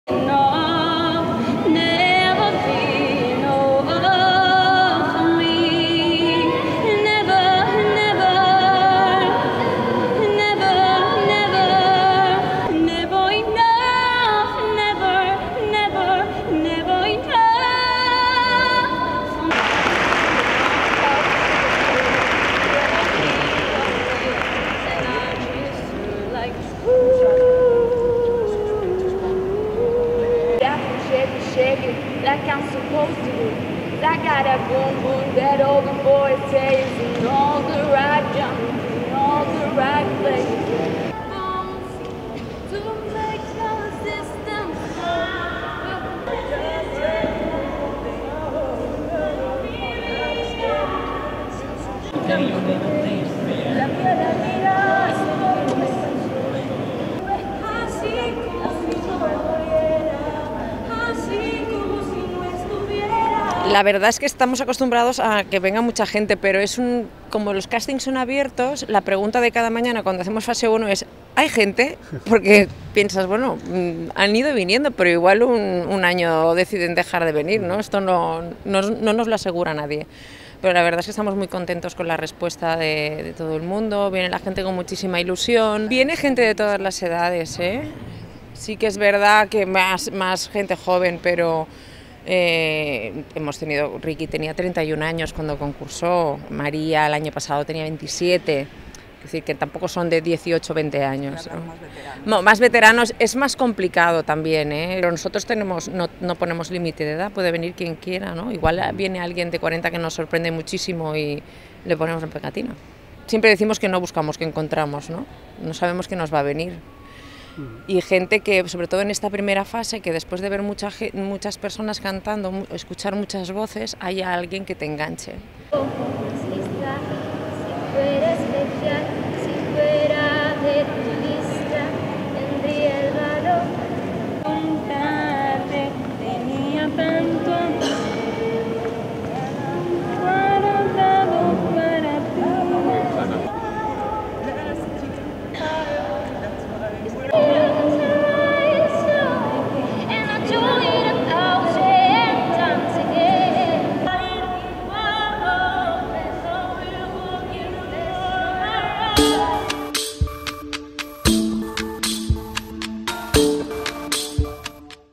Nos colamos en el casting de OT
El casting de Operación Triunfo llega a Santiago. Las pruebas de selección para el programa se iniciaron el 7 de octubre en Barcelona y terminarán el 5 de noviembre en Madrid. La cita de Santiago será la única en Galicia.